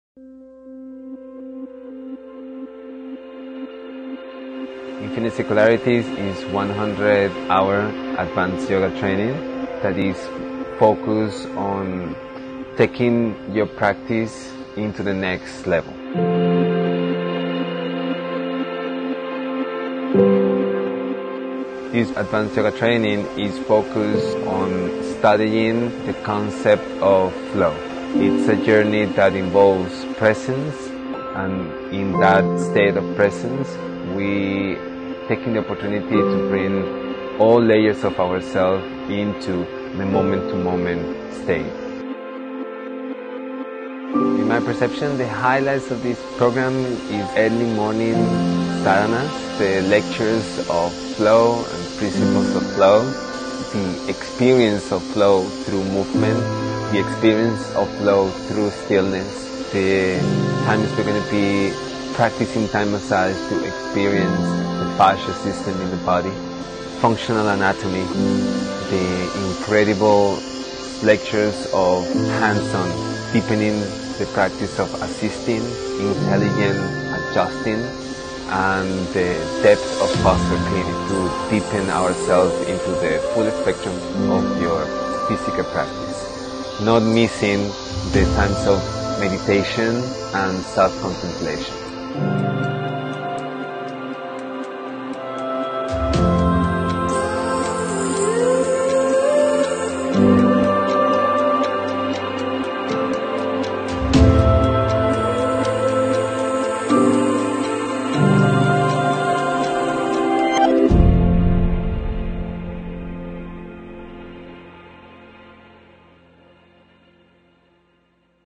Infinite Circularities Carlos Romero Livin Inspired 100HR Yoga Teacher Training The Yoga Barn Bali
Our bodies play a huge role in the accomplishment of our goals and our overall happiness.  Embodiment... is about tuning into our physical bodies and learning ways to support our health and well-being so that we can do what we need to do, and enjoy the journey...

100hr Yoga deepening/training focus on integration & explore different forms of movements and practices that will open up a deeper sense of connection & freedom within as a doorway to stimulate more flow on a mental physical emotional physiological & energetic level and activate  the power of self healing.